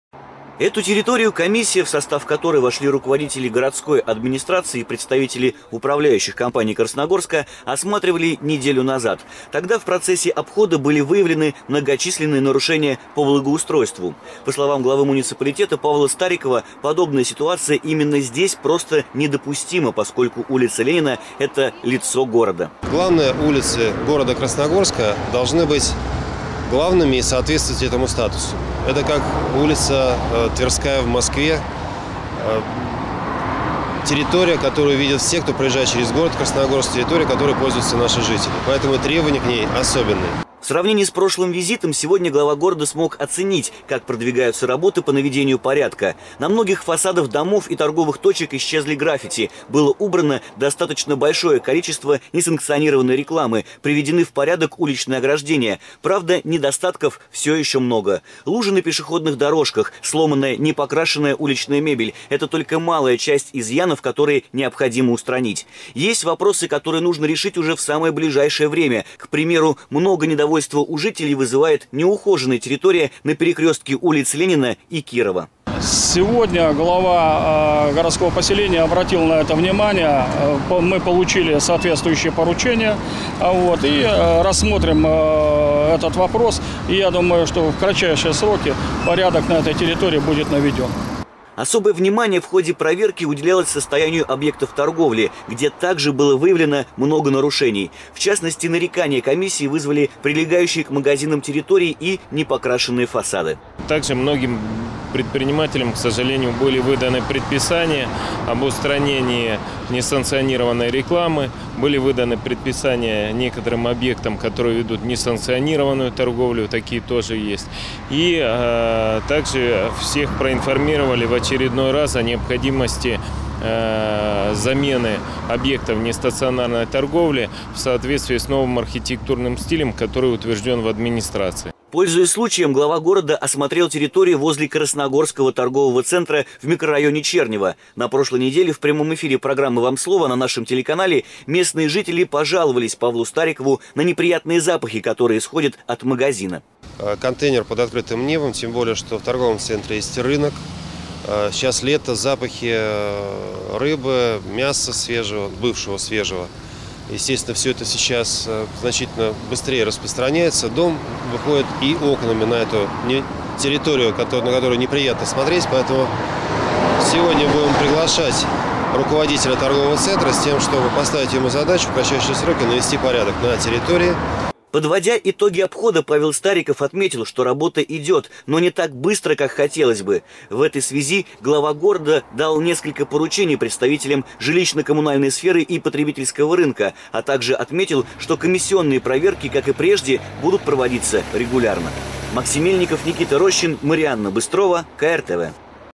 КРТВ. Глава города провёл выездное совещание
Очередное выездное совещание по вопросам благоустройства провел глава городского поселения Красногорск Павел Стариков. На этот раз комиссией был осуществлён рейд по  микрорайону «Чернево-1». В ходе осмотра выявлен ряд нарушений и замечаний по ненадлежащему содержанию пешеходной части и территорий прилегающих к объектам торговли. Для наведения порядка глава дал недельный срок